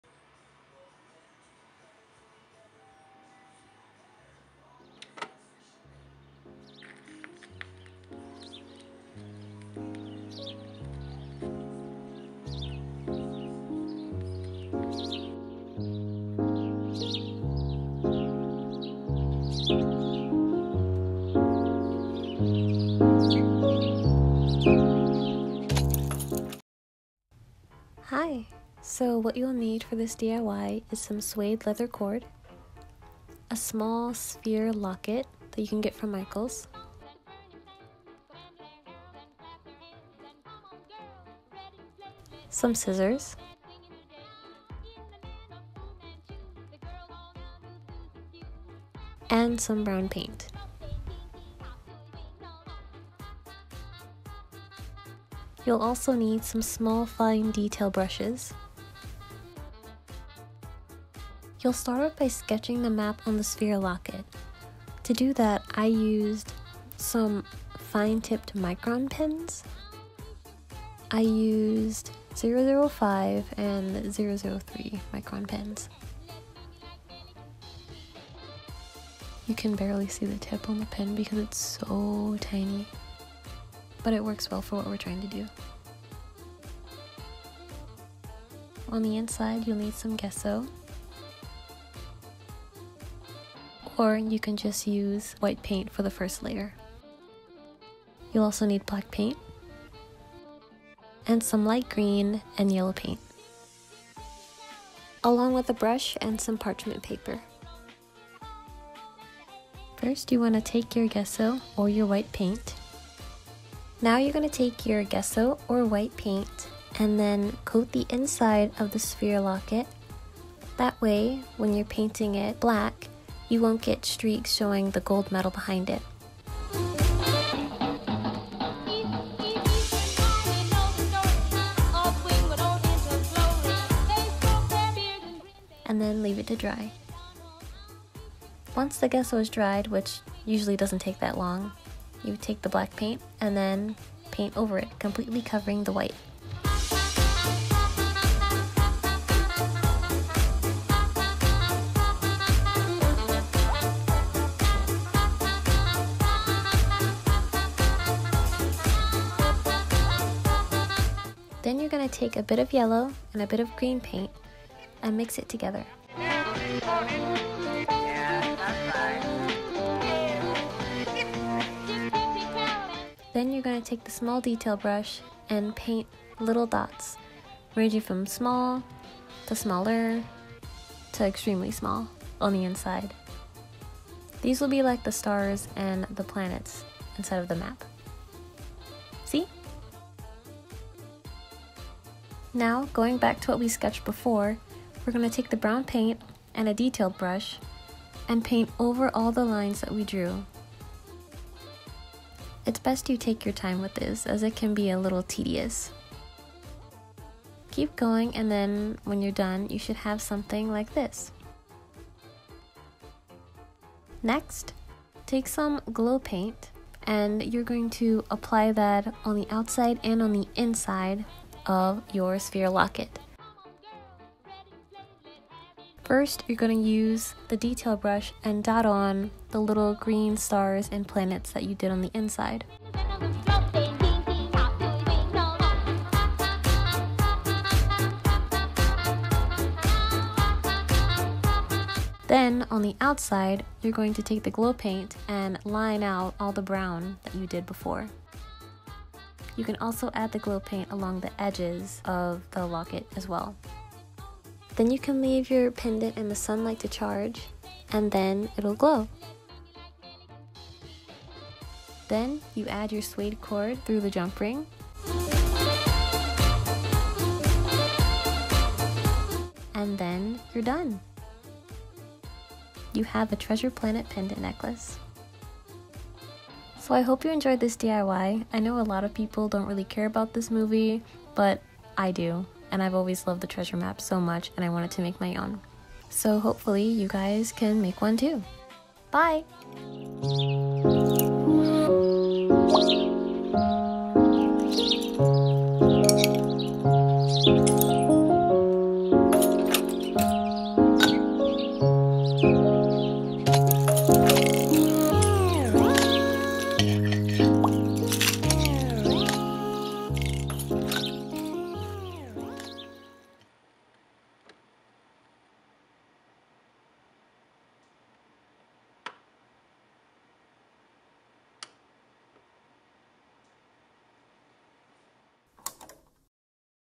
diy treasure planet pendant necklace
Hi! I decided to make a little diy of the map from the Disney movie Treasure Planet. I know its a pretty forgotten movie but its one of my favorites. I hope you like it!

☞ Detail Brushes |

✿ Depop: nicofawn